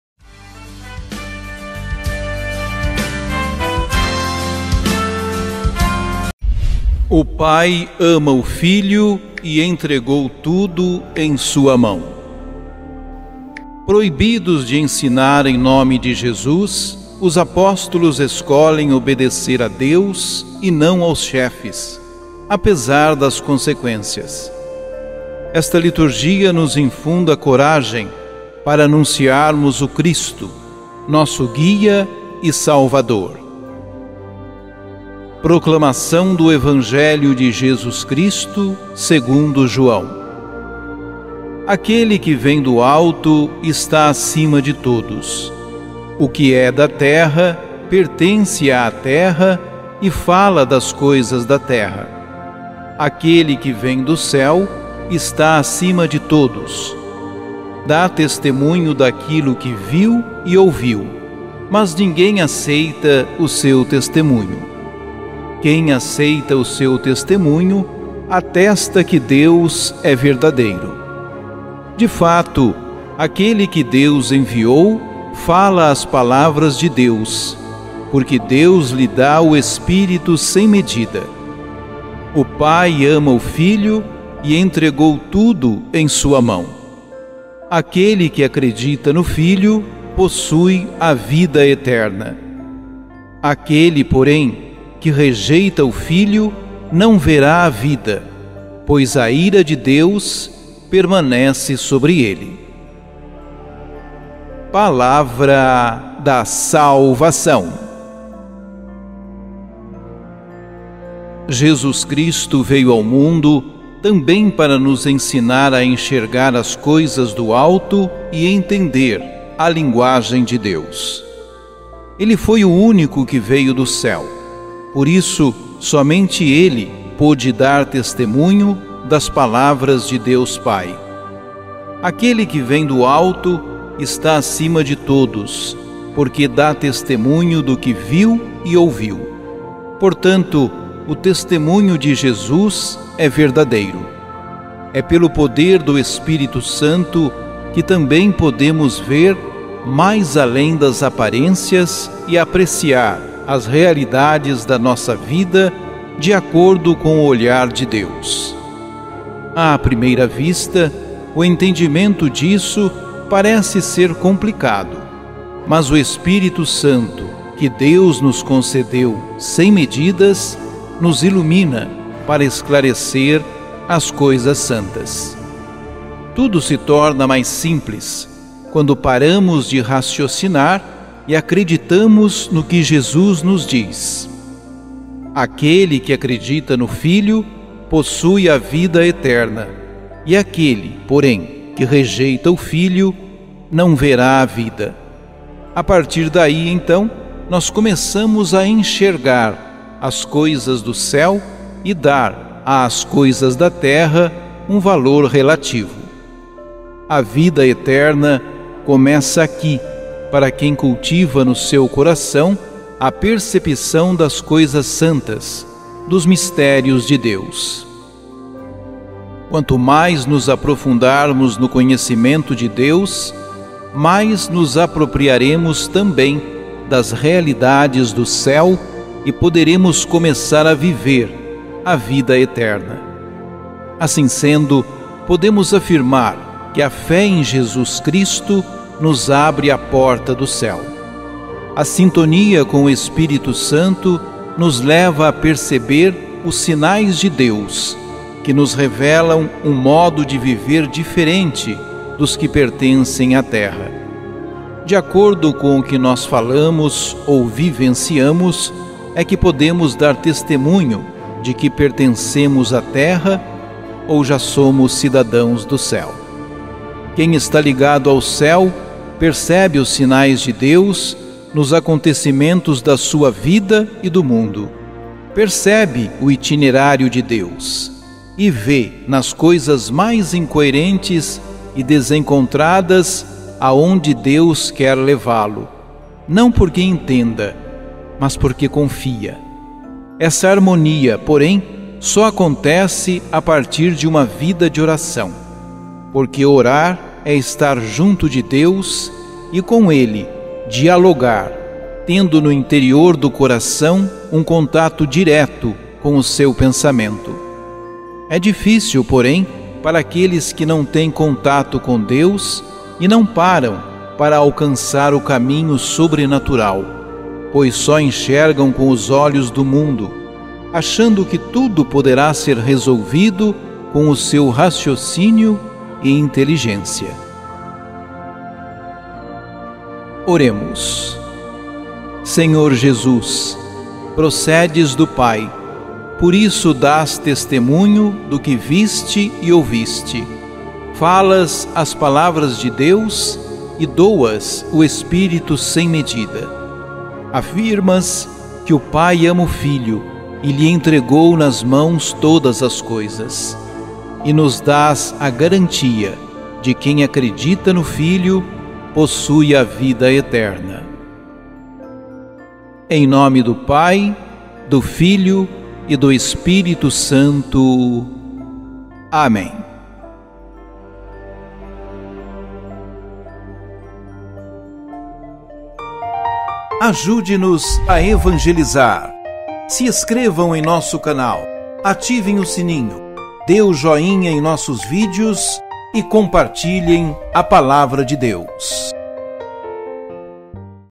EVANGELHO DO DIA 11/04/2024 COM REFLEXÃO. Evangelho (João 3,31-36)
"O Pai ama o Filho e entregou tudo em sua mão."

31 "Aquele que vem do alto está acima de todos.
O que é da terra, pertence à terra e fala das coisas da terra.
Aquele que vem do céu está acima de todos.
32 Dá testemunho daquilo que viu e ouviu, mas ninguém aceita o seu testemunho.
33 Quem aceita o seu testemunho atesta que Deus é verdadeiro.
34 De fato, aquele que Deus enviou fala as palavras de Deus,
porque Deus lhe dá o espírito sem medida.
35 O Pai ama o Filho e entregou tudo em sua mão.
36 Aquele que acredita no Filho possui a vida eterna.
Aquele, porém, que rejeita o Filho não verá a vida,
pois a ira de Deus permanece sobre ele".